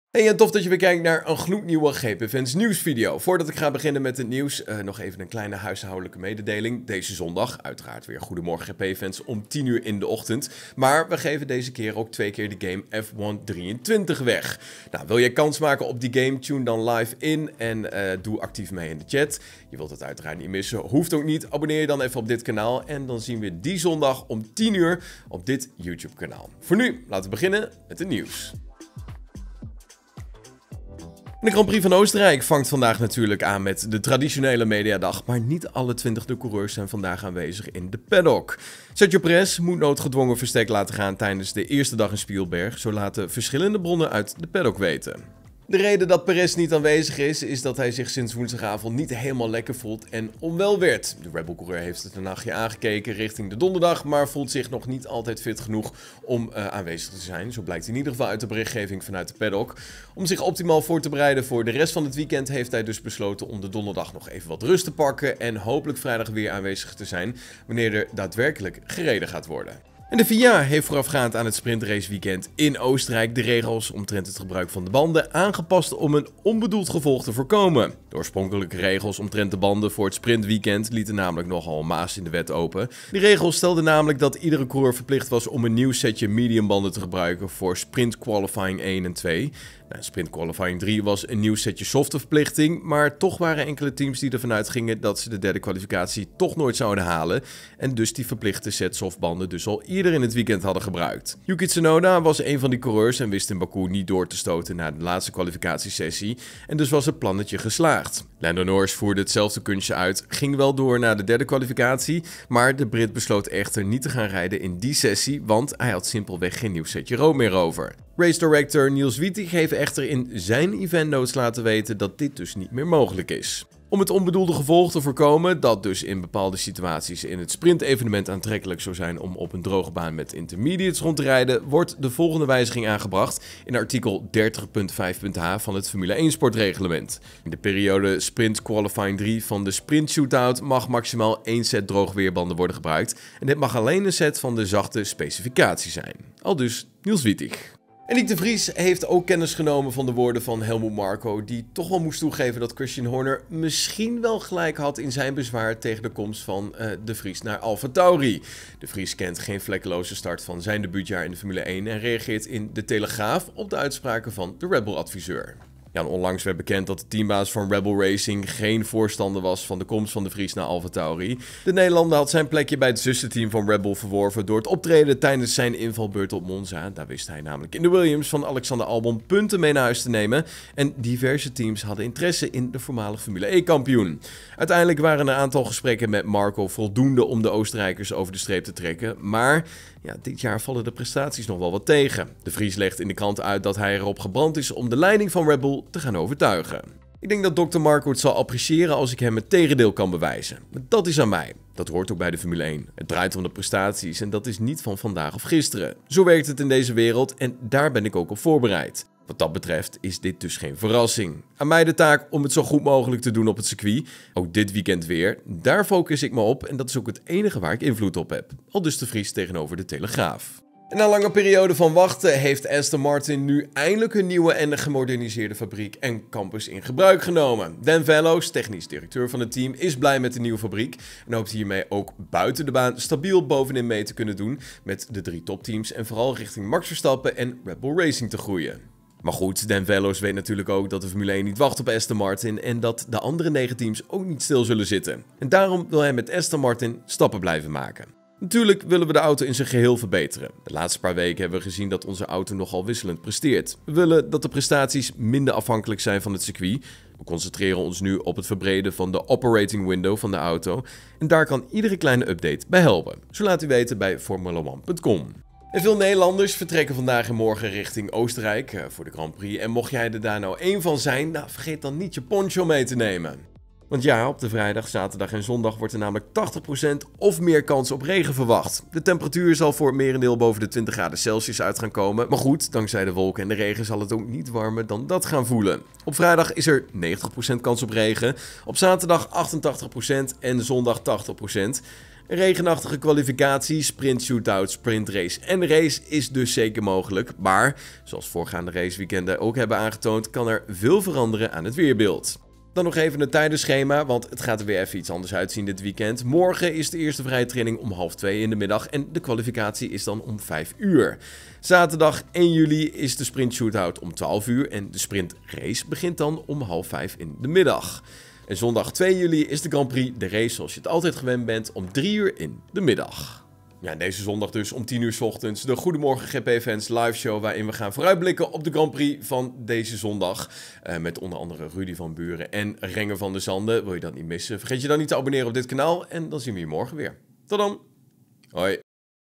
Pérez mist eerste dag Oostenrijk, FIA wijzigt regels bandengebruik tijdens F1 Sprint | GPFans News
De Grand Prix van Oostenrijk vangt op donderdag aan met de traditionele mediadag, maar niet alle twintig coureurs zijn op donderdag aanwezig in de paddock. Sergio Pérez moet noodgedwongen verstek laten gaan tijdens de eerste dag in Spielberg, zo laten verschillende bronnen uit de paddock weten. De FIA heeft voorafgaand aan het sprintweekend in Oostenrijk de regels omtrent het gebruik van de banden aangepast om een 'onbedoeld gevolg' te voorkomen.

Montage door: Ronan Postulart